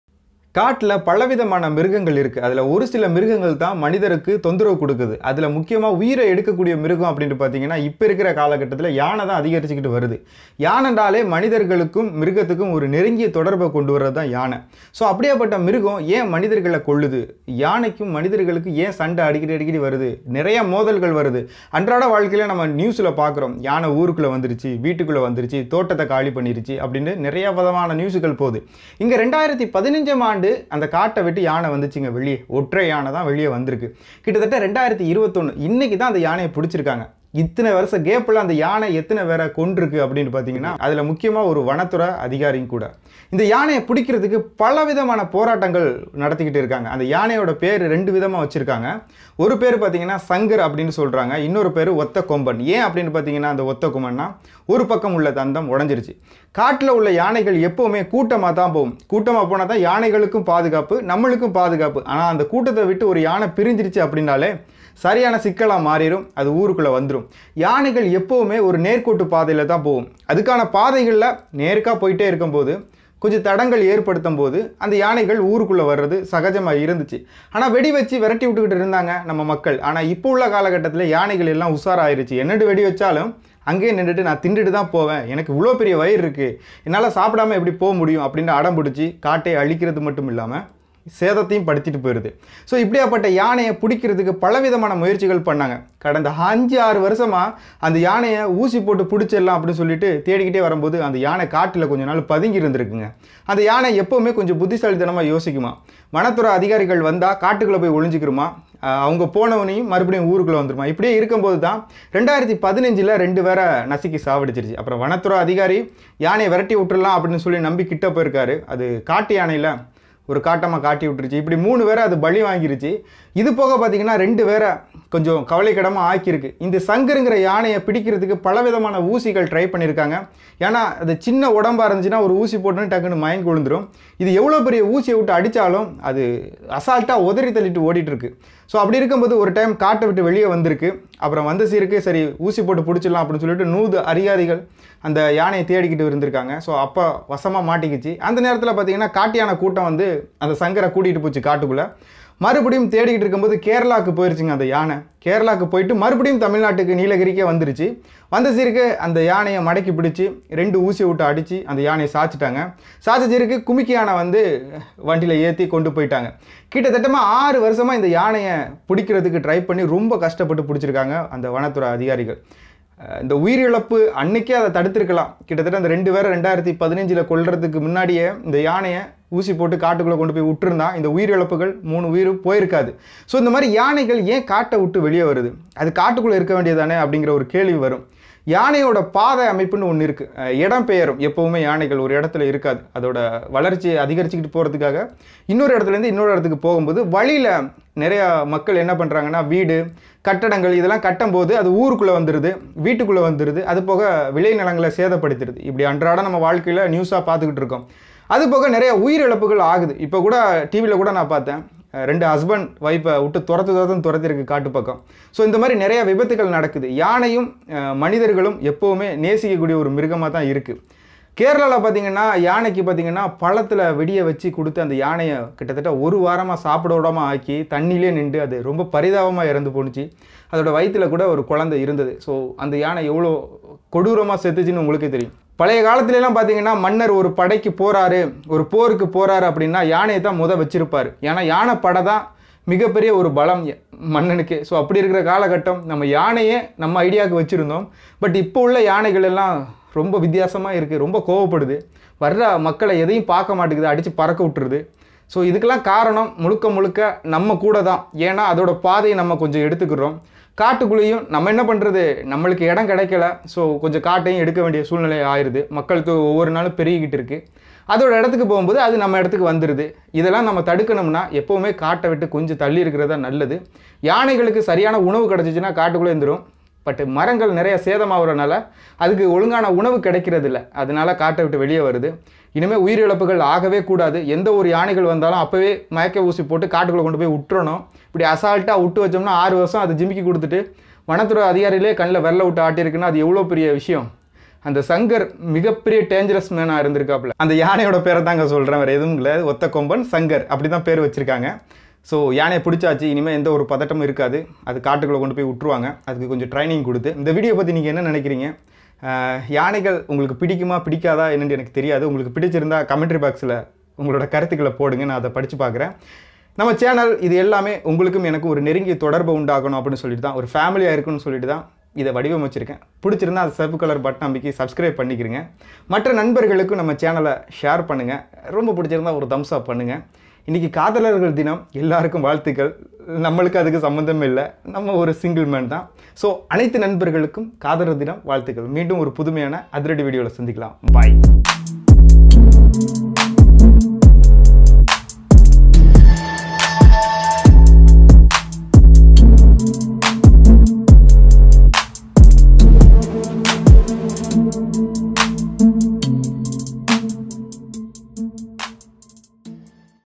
Yanai அட்டகாசம் சங்கர் யானை Elephant today update video Tamil
Elephant அட்டகாசம் சங்கர் யானை Elephant today update video Tamil...
வனத்துறை அதிகாரிகள் பல மணி நேரம் போராட்டம் பிறகு யானை பிடிபட்டது...